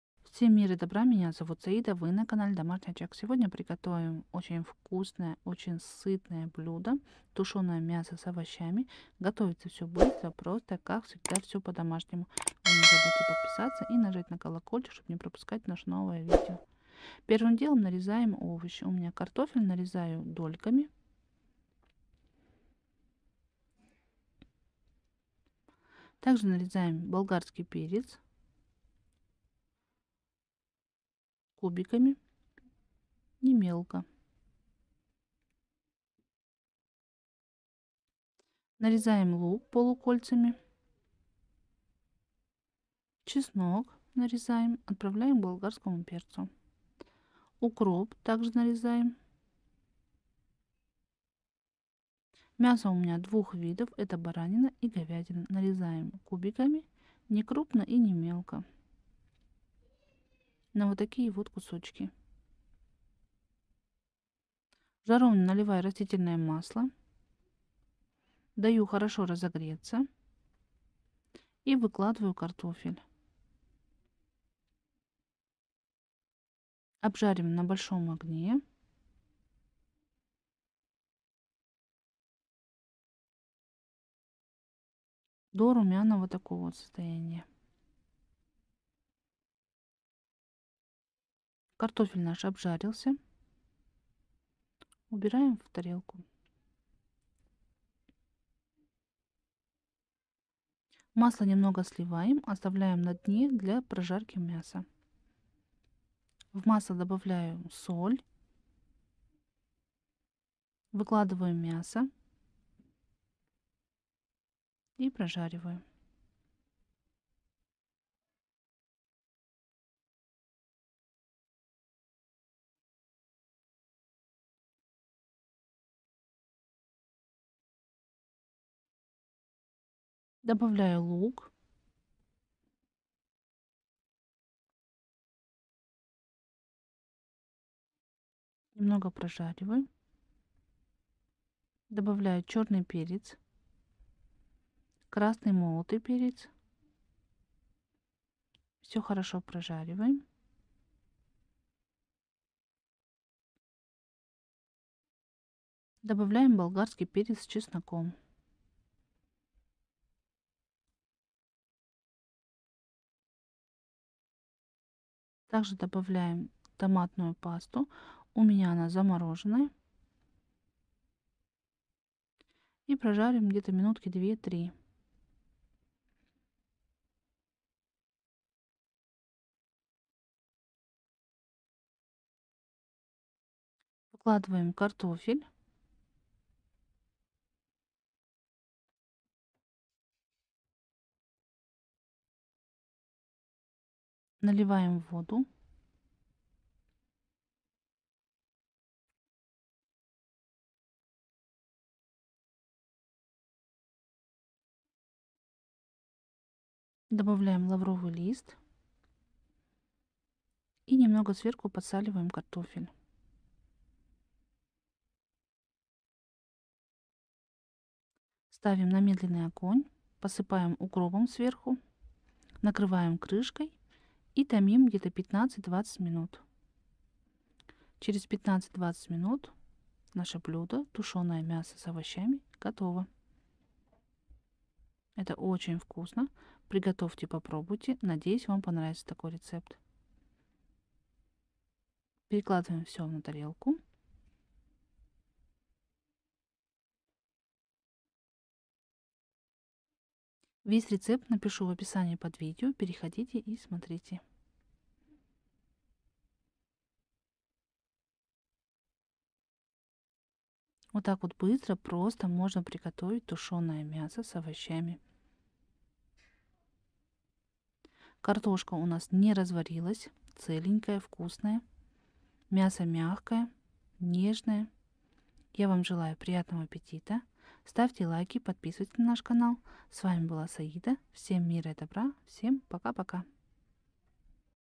ТАК ПРОСТО Я ЕЩЁ НЕ ГОТОВИЛА. ТУШЁНОЕ МЯСО С ОВОЩАМИ😋 ОЧЕНЬ ВКУСНЫЙ УЖИН meat with vegetables
Всем мира и добра меня зовут Саида и вы на канале Домашний очаг ♥️
сегодня приготовим самое простое но очень вкусное блюдо .тушёное мясо с овощами . готовится так просто, получается очень вкусно.приготовьте попробуйте надеюсь что вам понравится этот рецепт 😋👍

рецепт 📝
чеснок -4-5зуб.
укроп
мясо
томатная паста -2 с.л.
вода -1.5 стакана
соль.
перец молотый черный
красный перец молотый
лавровый лист